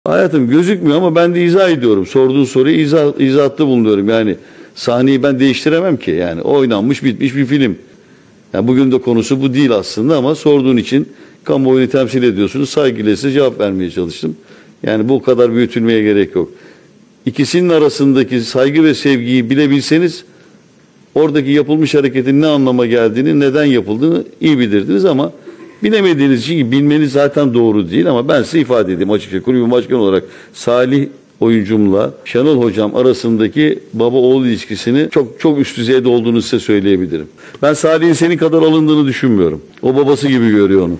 Ahmet Nur Çebi'den Salih Uçan açıklaması: Salih'in senin kadar alındığını düşünmüyorum
Beşiktaş Başkanı Ahmet Nur Çebi, Yukatel Adana Demirspor'a deplasmanda 4-2 kaybedilen maçın ardından taraftarların yönelttiği istifa çağrılarına cevap verdi. Çebi ayrıca Süper Lig'e getirilmesi gündemde olan play-off sistemine yeşil ışık yaktı. Salih Uçan ile Şenol Güneş arasındaki gerginliğe dair gelen ilk soruya yanıt vermesinin ardından sorunun yinelenerek konunun uzatılmasına kızan Çebi beklenmedik bir çıkışa imza attı.